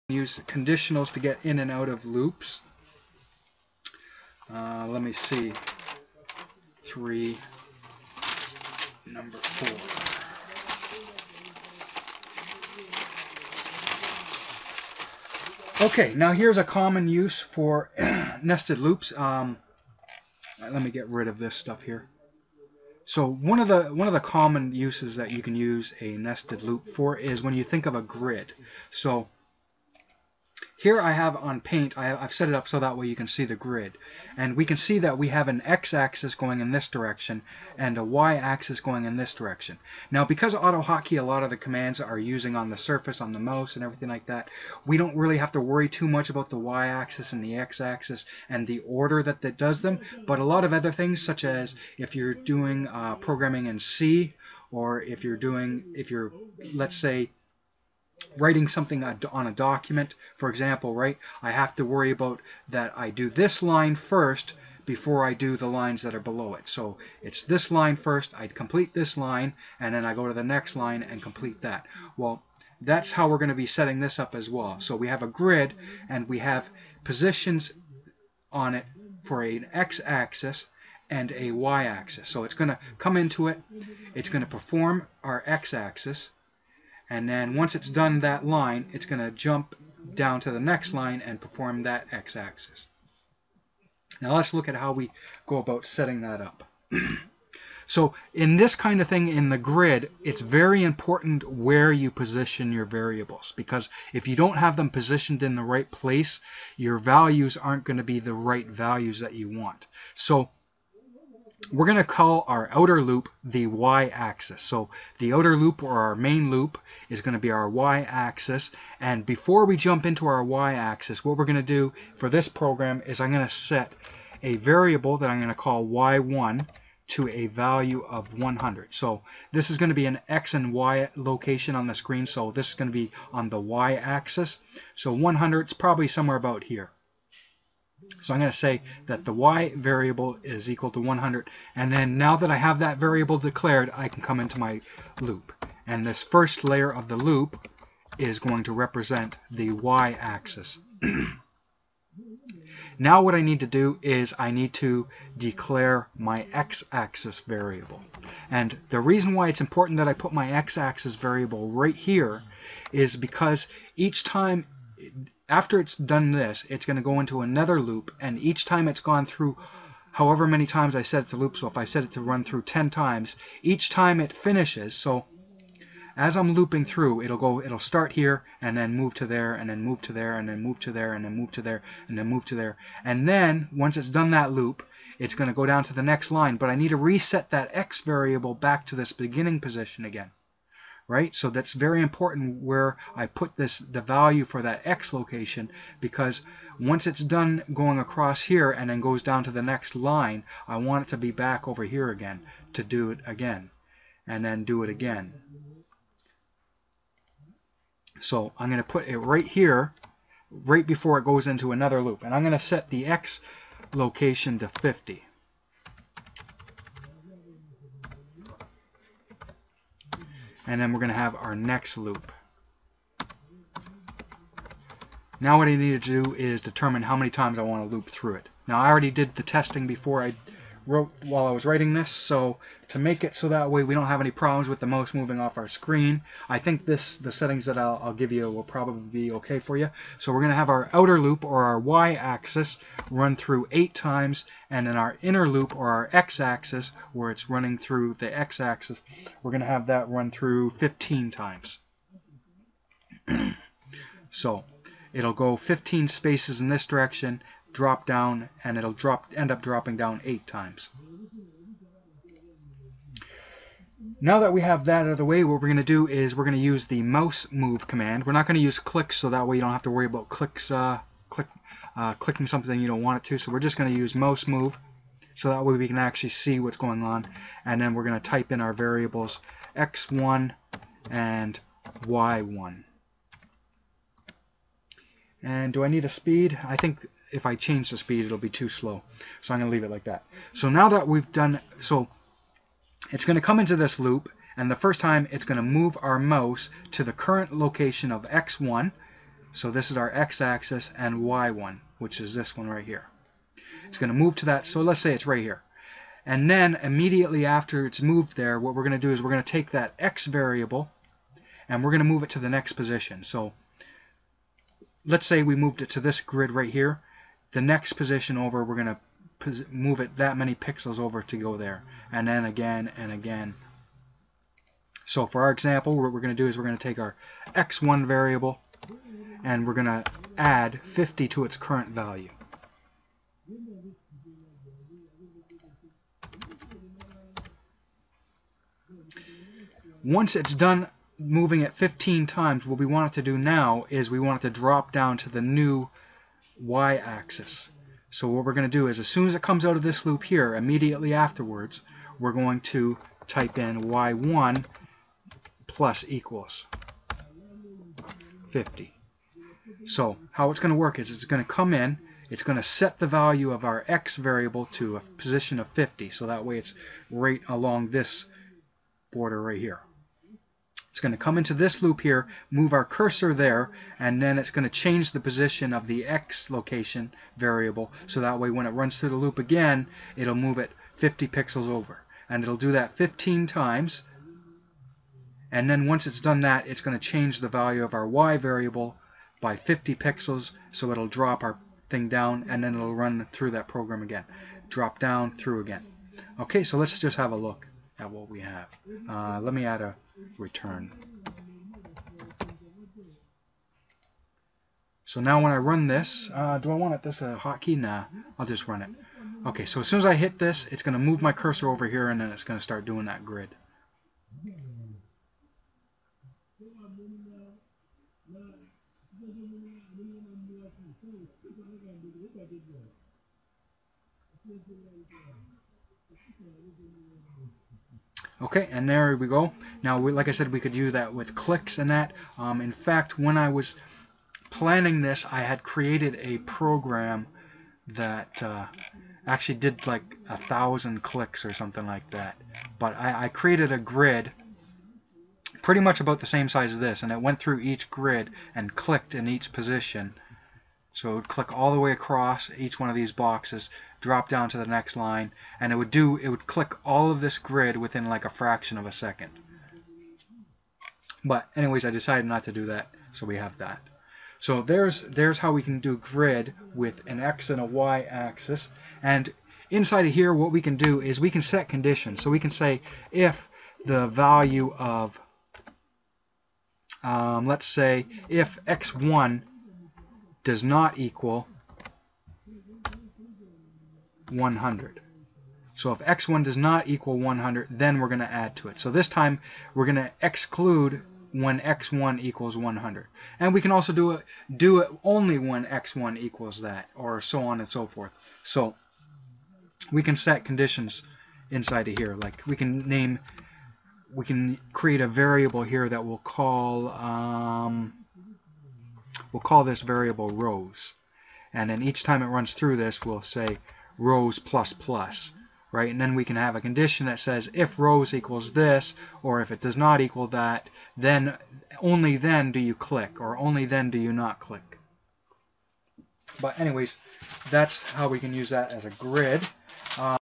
AutoHotkey Logic Tutorial # 3-3 Nested Loops
Support the creation of these tutorials by becoming a Patron

This tutorial explores how to use nested loops in scripting logic.

This tutorial is dedicated to:
paindoc25
Jun Ng